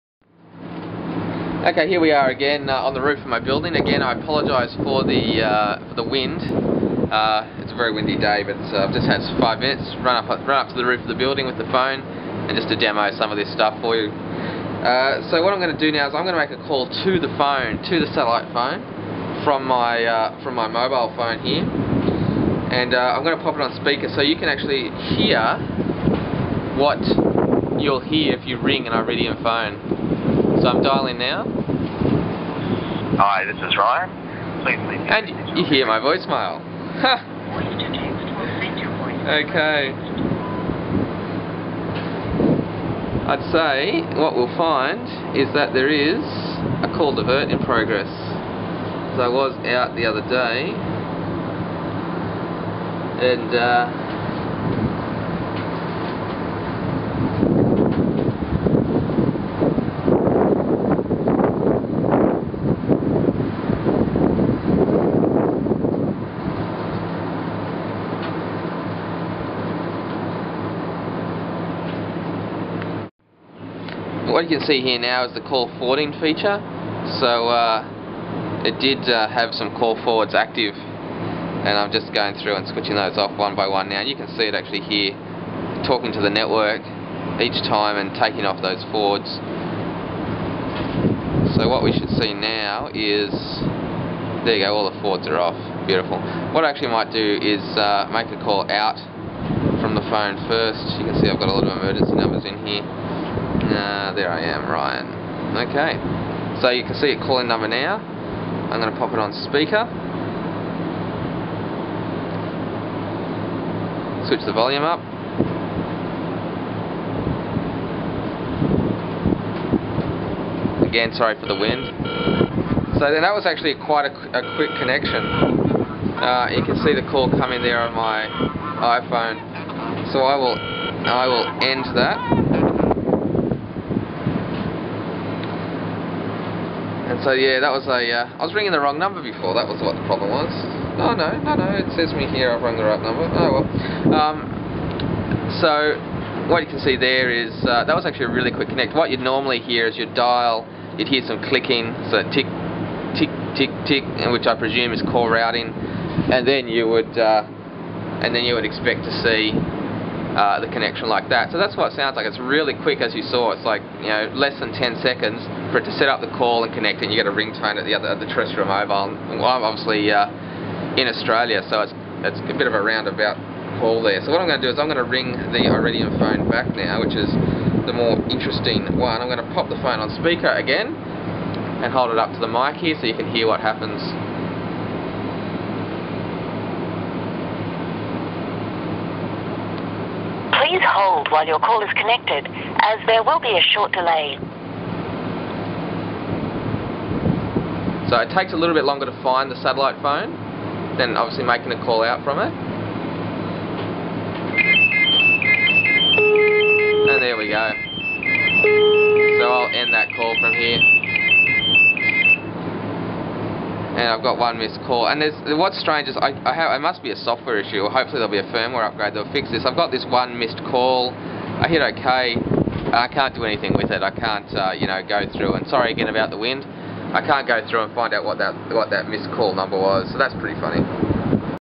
Iridium 9555 Making and Receiving Calls (Part 2 of 3)
By request a video showing calls being made into and from an Iridium handset, the phones are on speaker so you get to hear what the call set up sounds like. Sorry for the wind noise and the glare.